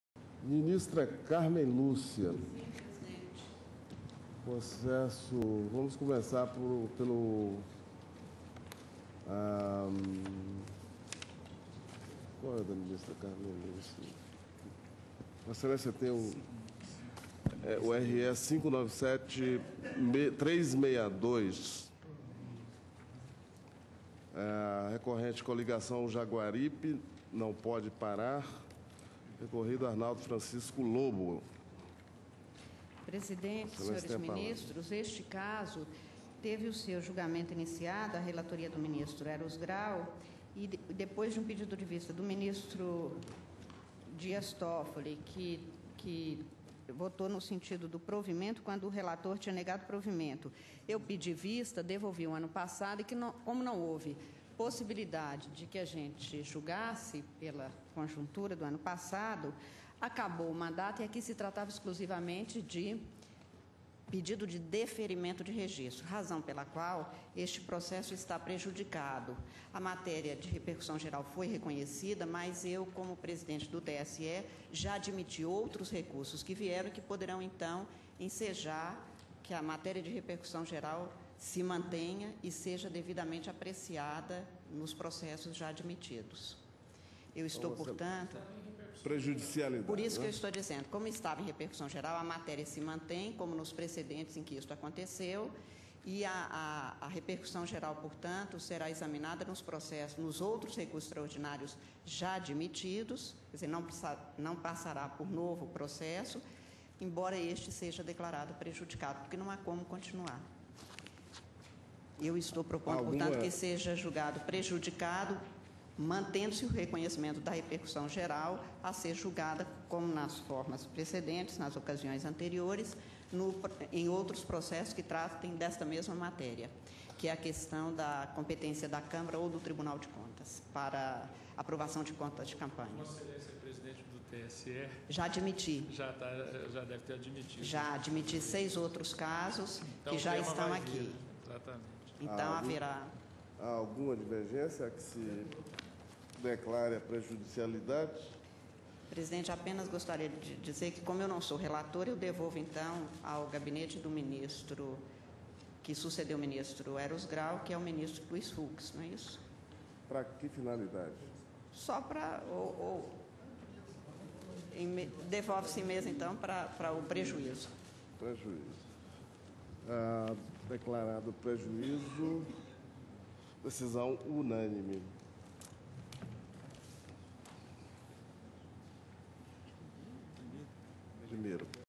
Pleno - STF irá analisar eficácia de parecer de TCE sobre contas de prefeito
O Plenário do Supremo Tribunal Federal (STF), no dia 21 de fevereiro de 2013, julgou prejudicado o Recurso Extraordinário (RE) 597362 em que se discute se a demora ou ausência de manifestação da Câmara Municipal determina ou não a aprovação tácita do parecer prévio de Tribunal de Contas estadual ou Tribunal de Contas municipal, onde houver, sobre as contas de um prefeito. Apesar de esse recurso ter ficado prejudicado, o tema tem repercussão geral reconhecida e, portanto, será discutido quando a Corte analisar outros processos sobre o mesmo assunto que aguardam julgamento.